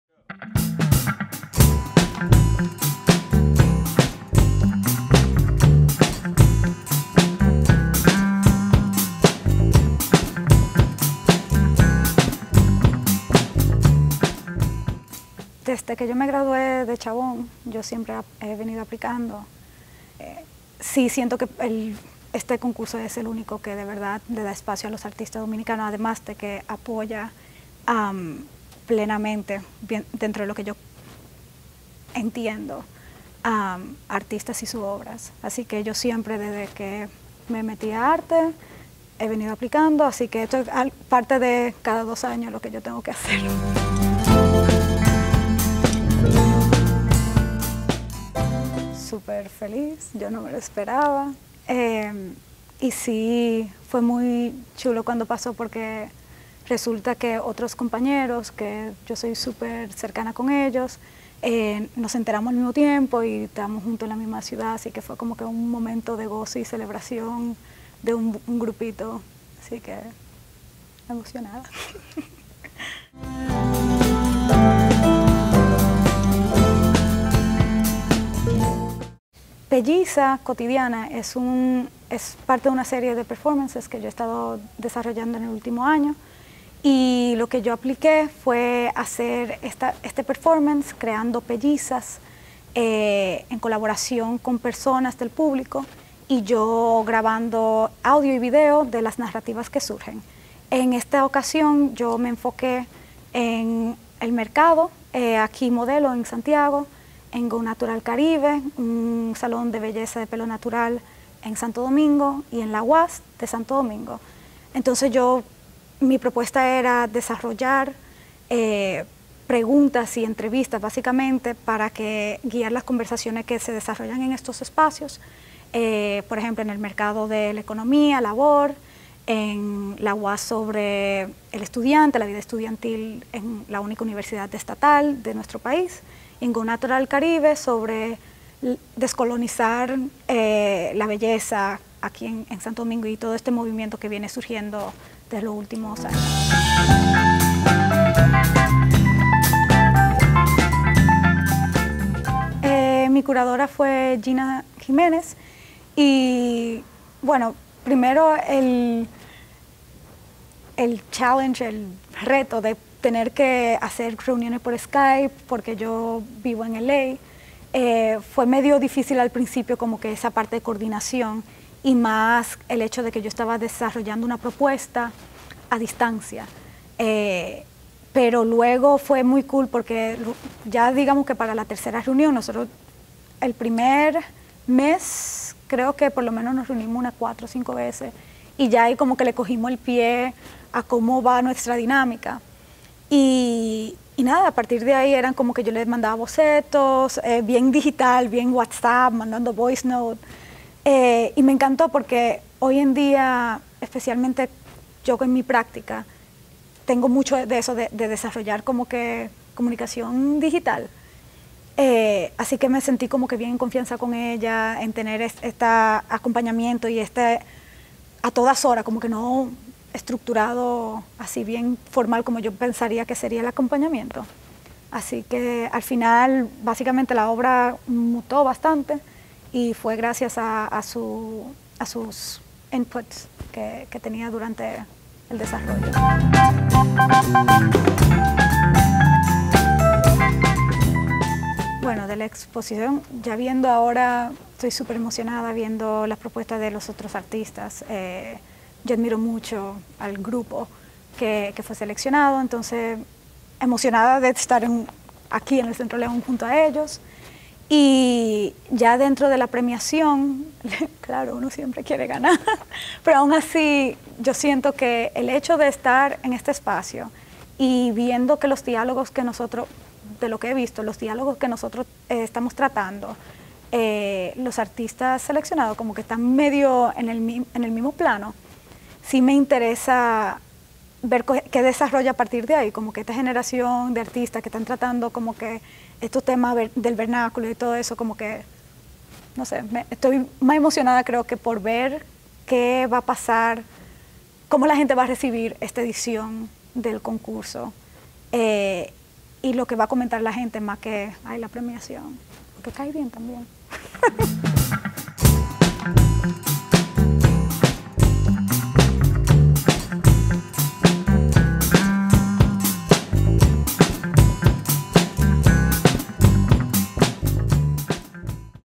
Centro León. Entrevista a Gina Goico
Artista seleccionada para el 26 Concurso de Arte Eduardo León Jimenes. Santiago, República Dominicana. Octubre 2016.